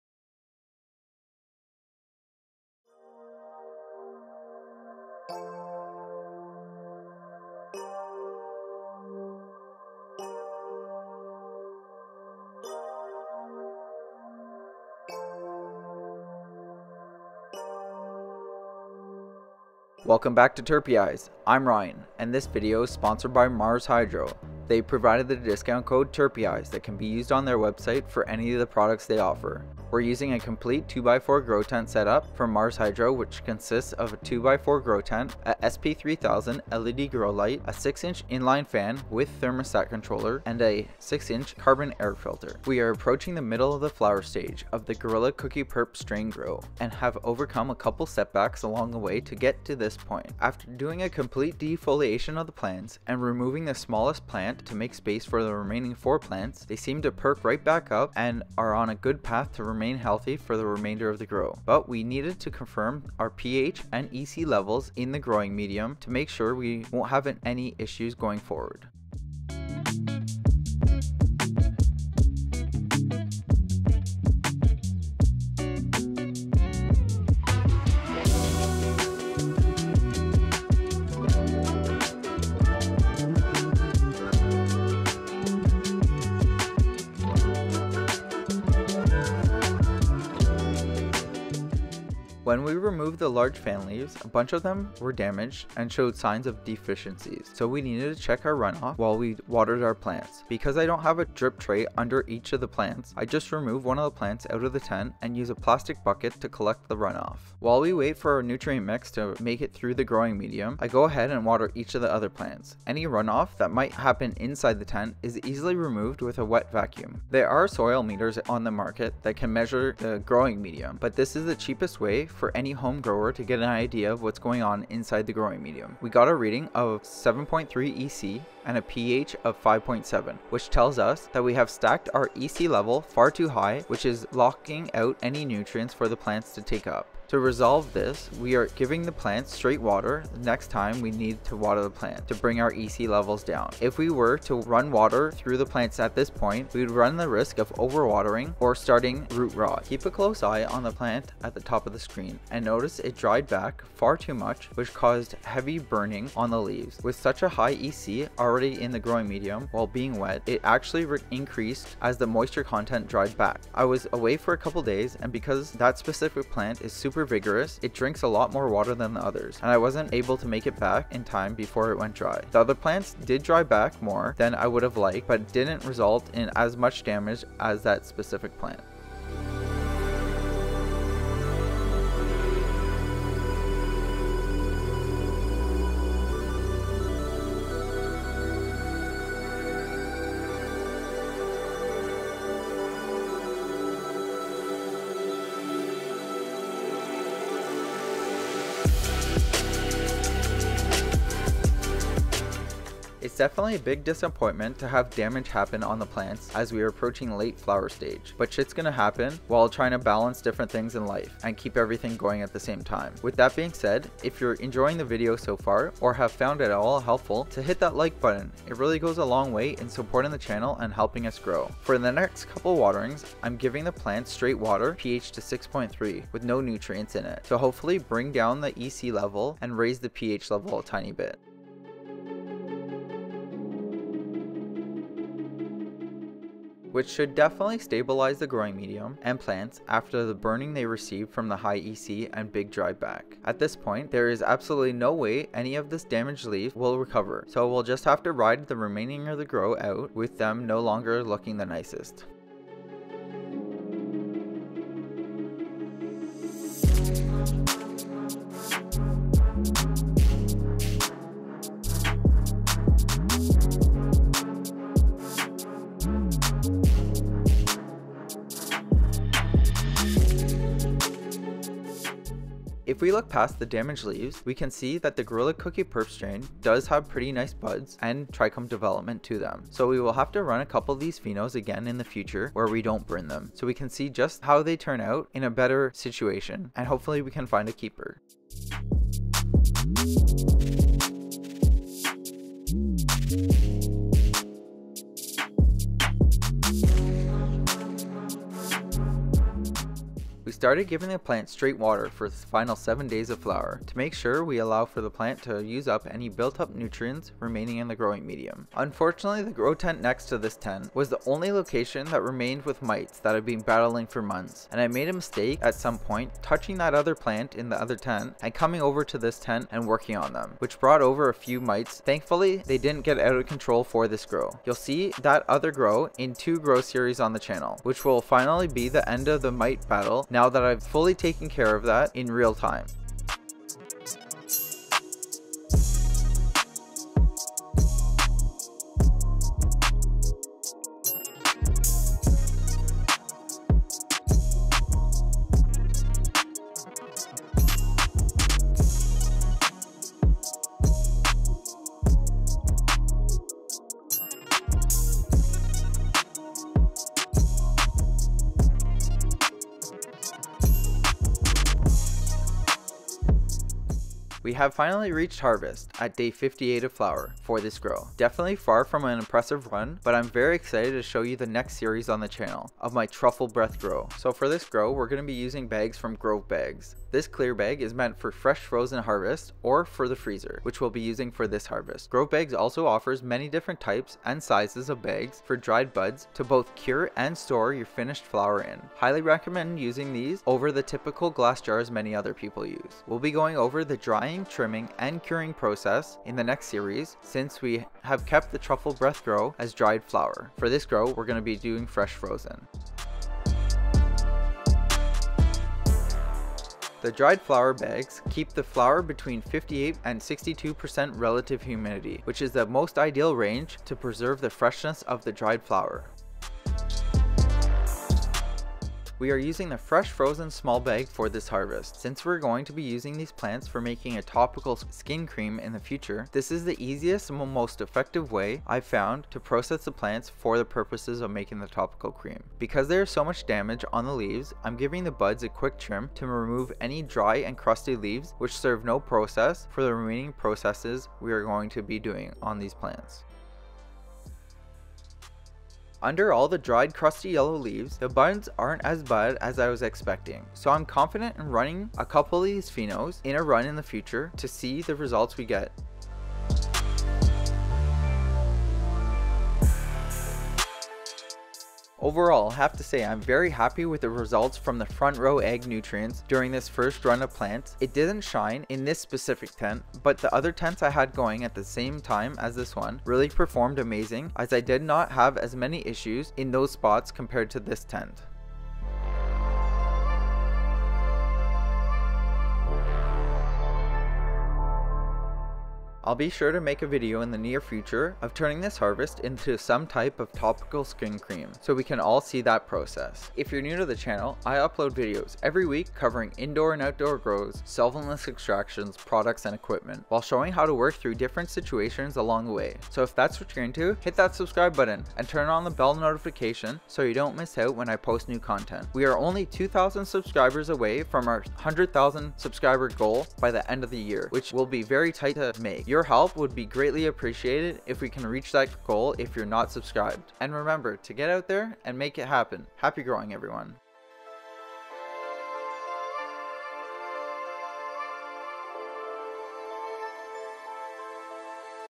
Measuring RUNOFF during FLOWER STAGE and HARVESTING THE PLANTS
We take EC and PH readings of out runoff after a heavy watering with out nutrient mix from front row ag nutrients. Along with cover the remainder of the flower cycle which includes harvesting the 4 Gorilla Cookie Purp Plants.

Timelapse Camera
Main/b-roll Camera
Camera Lense
Big Tripod
Small Tripod
Studio Lights